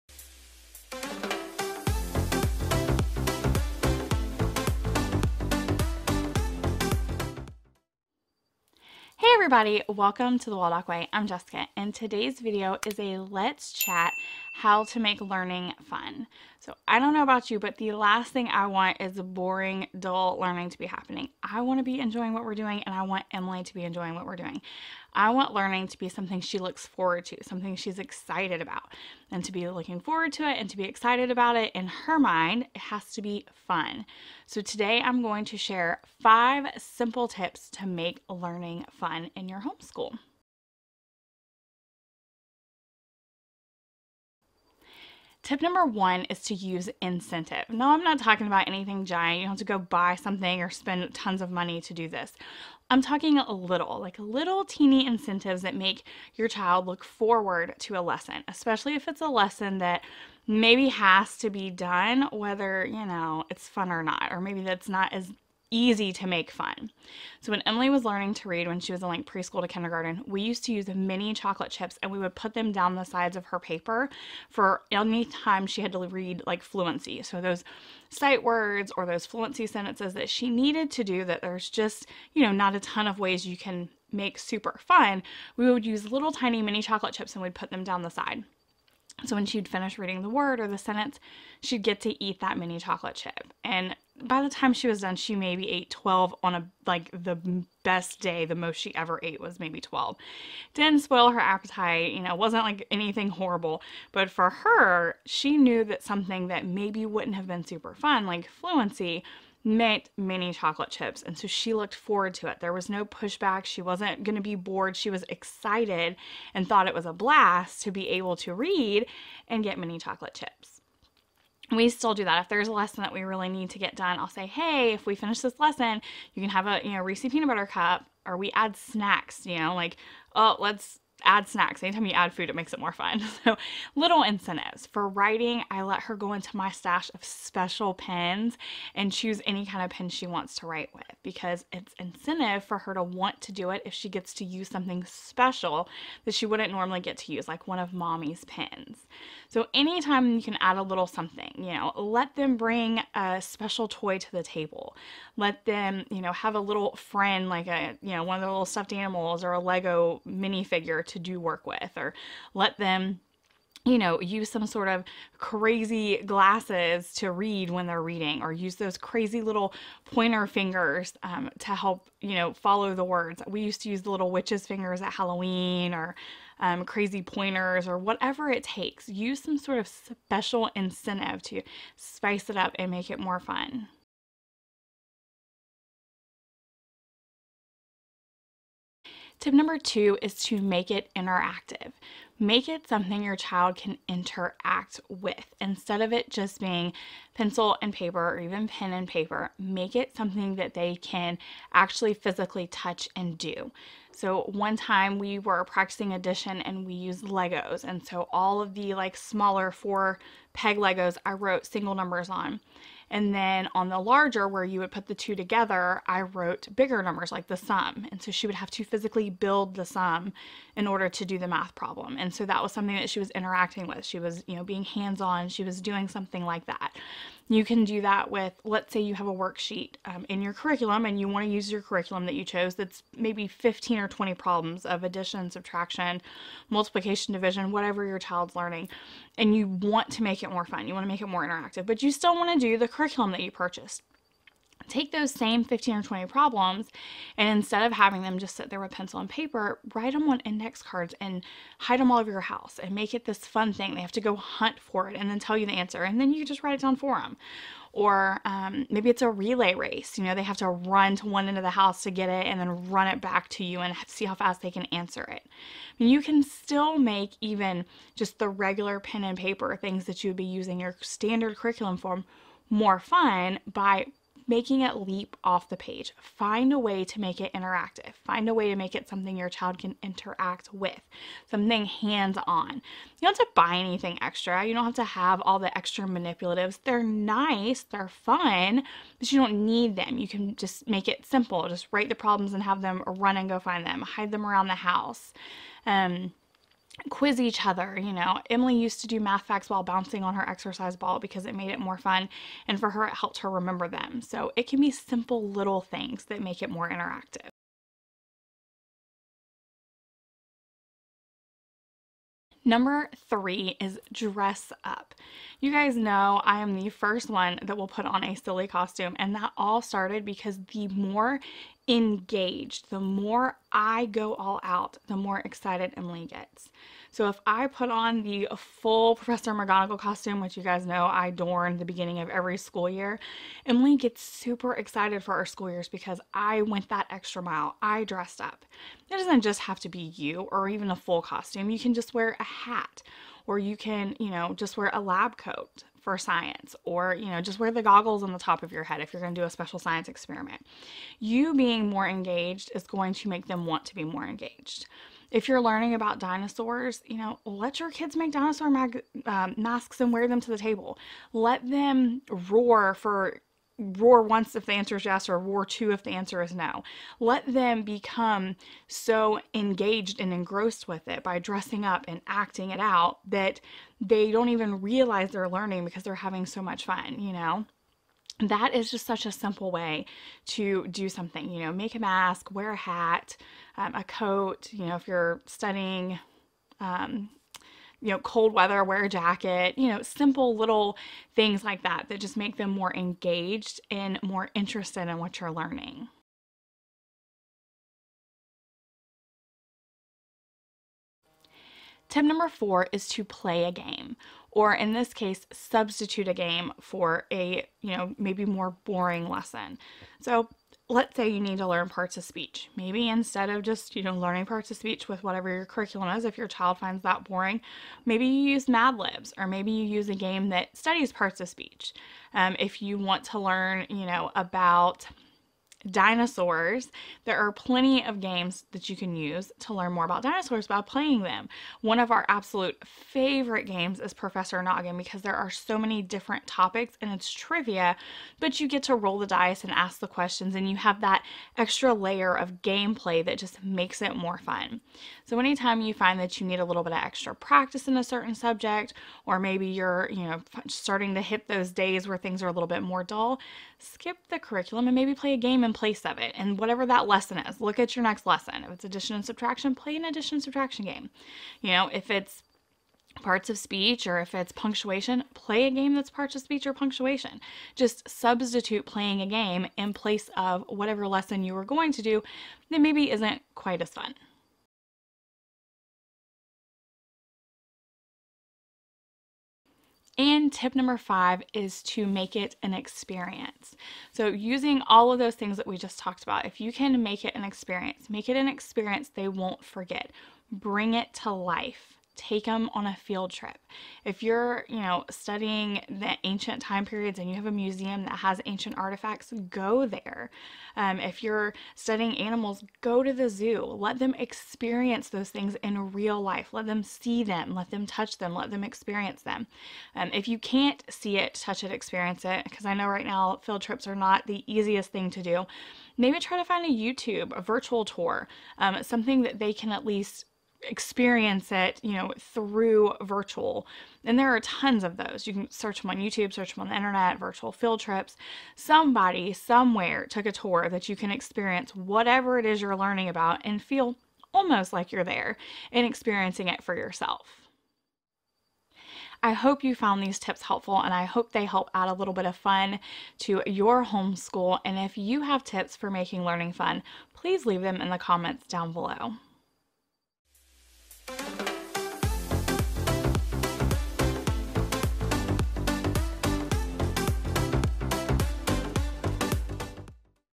Five Tips to Make Learning More Fun!
Who wants a boring homeschool? I don't! I am sharing my 5 simple tips for making learning fun in your homeschool. These tips are all things you can start doing right away!

Ring Light - Neewer
Softbox Lights - Neewer